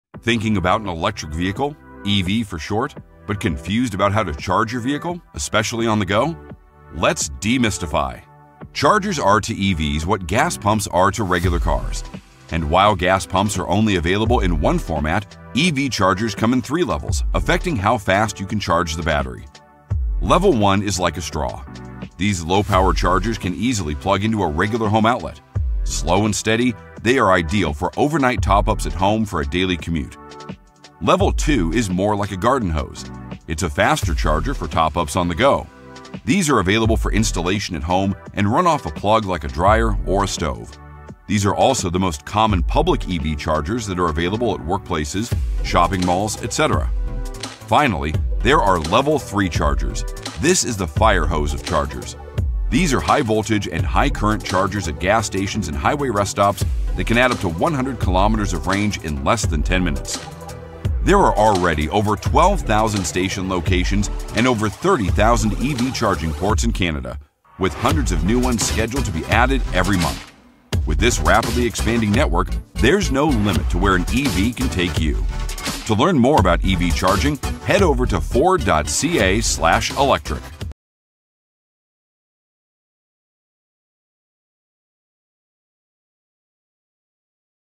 EV Education | Understanding the 3 Levels of EV Charging
There are 3 levels of EV charging that affect how quickly an EV can be charged.  Let’s look at how they differ, and where they are available.